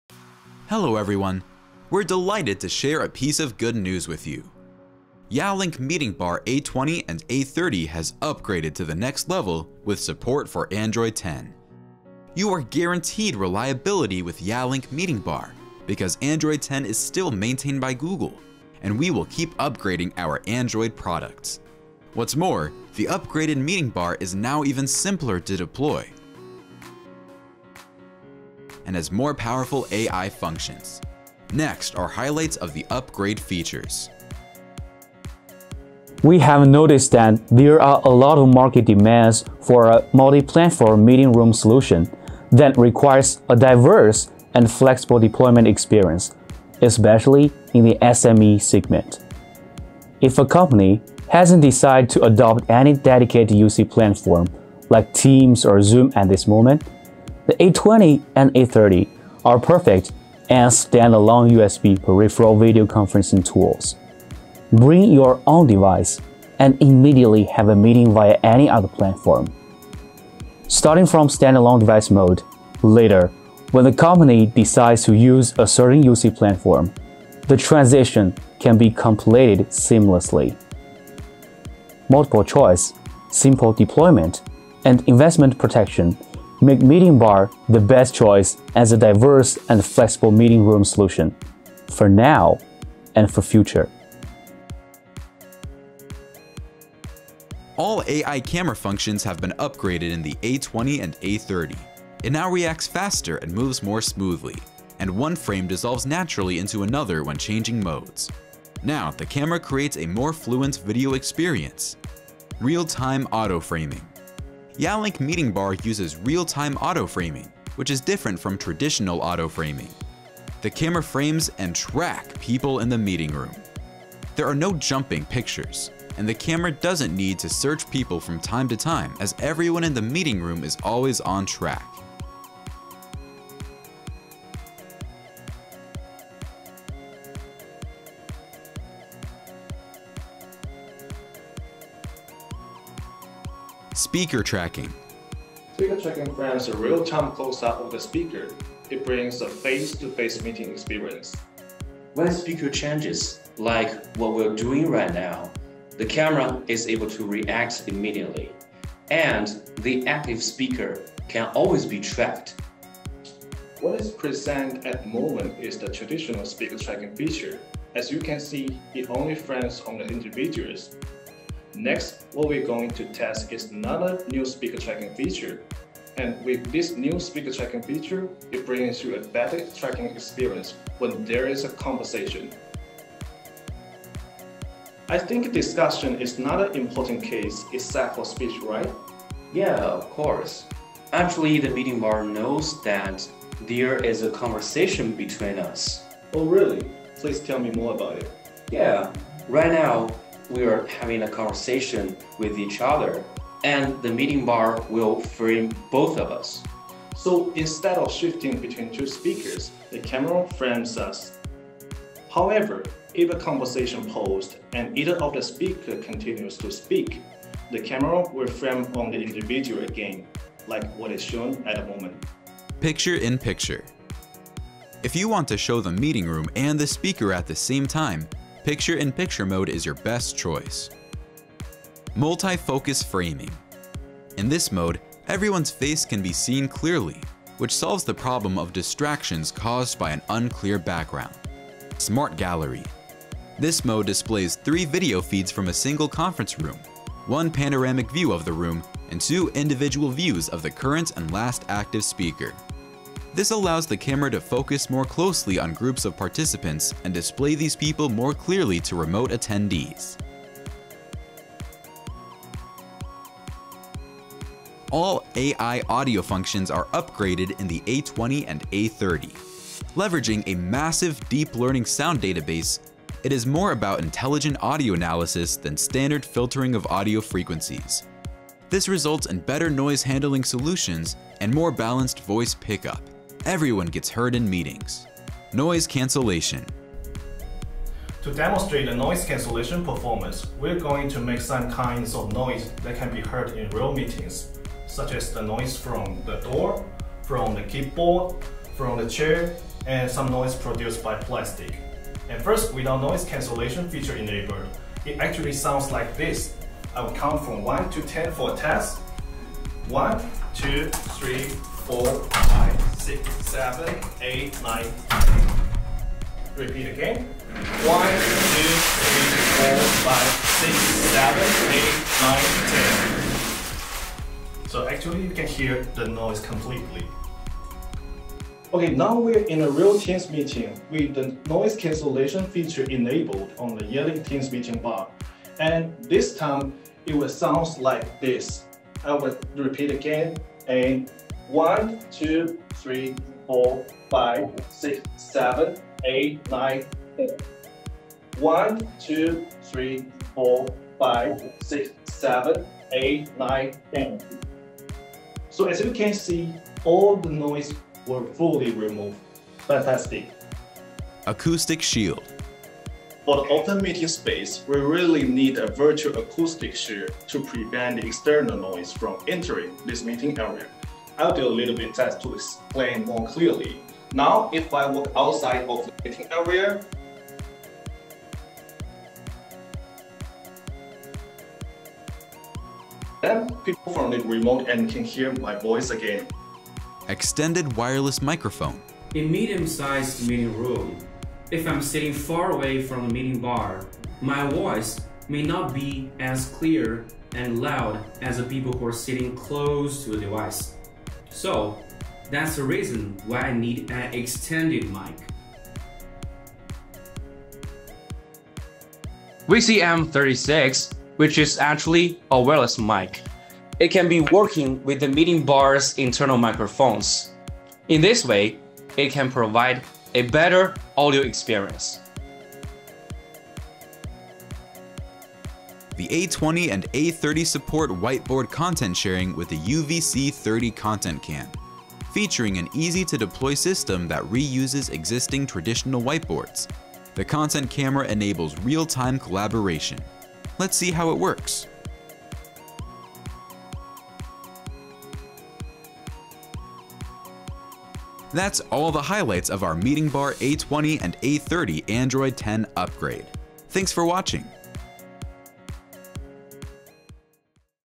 Yealink MeetingBar Android 10 Upgrade Highlights
MeetingBar A20 A30 Upgrade Highlights
-Start with standalone device mode, future proof with native Teams/Zoom experience;
-Wireless experience on device mode;
-ALWAYS ON TRACK: real-time and precise framing and tracking;
-EVERYONE GETS HEARD: AI-enhanced noise cancellation;
-Expanding the room: extended wireless mic. VCM36 supported;
-Whiteboard content sharing with UVC30 (Zoom only).